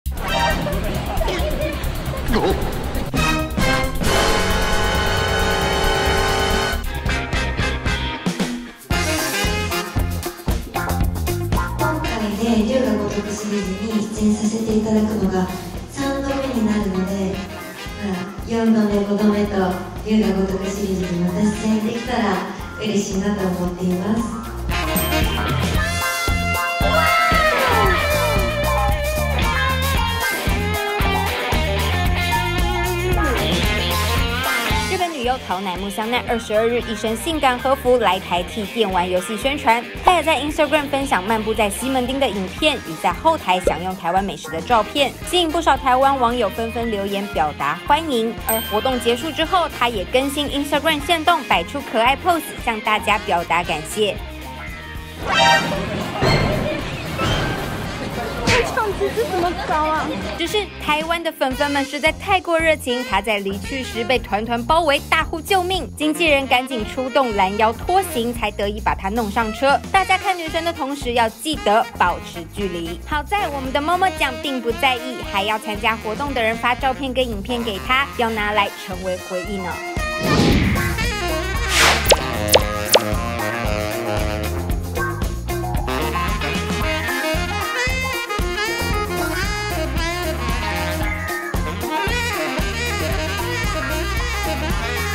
【撩星聞】畫面曝光！ 女優來台當街被攔腰拖上車 桃乃木香奈驚恐大呼「救命！」
日本女優桃乃木香奈22日一身性感和服，來台替電玩遊戲宣傳，她也在IG分享漫步在西門町的影片與在後台享用台灣美食的照片，只是沒想到活動結束後，因為粉絲太過熱情，出現令人驚恐的場面。